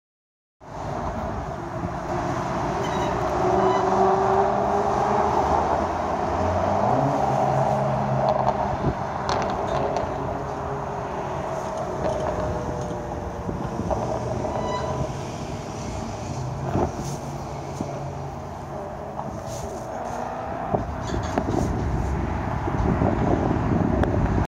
BMW F13 M5 Audi B7 RS4 Exotic 🚗
Accelerating Audi RS4 (B7) w exhaust sounds. Right behind is a (F13) M5 Coupe a muted V-8 exhaust. Watch on Verizon Fios ch 837, give a 👍.